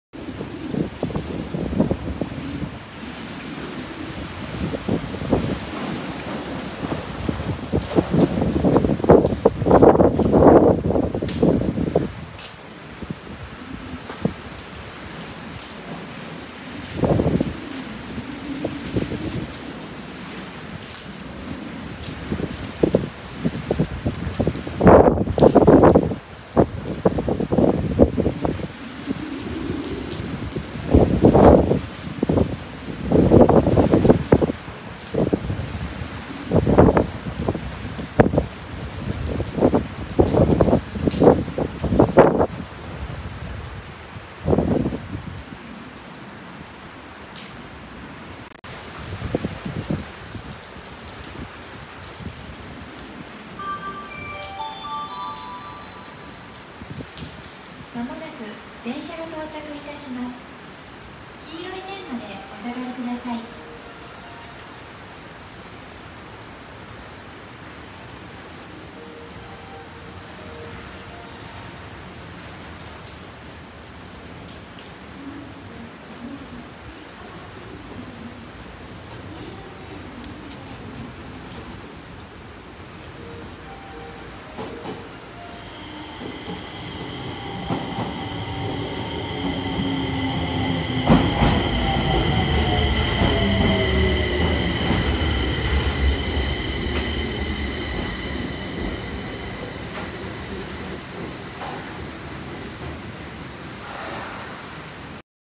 【vlog的なもの】ちょっと風雨が強かった一日【浜松】
こんにちは

今日は雨風ともに強かったですが、用事があったので外出せざるを得ませんでした。特別何かがあったわけではないですが、駅での待ち時間暇だったので、Vlog的な感じで日常の短い動画を出してみるのも面白いなと思い、ちょっとやってみました。

今後も気が向いたら出すかもしれません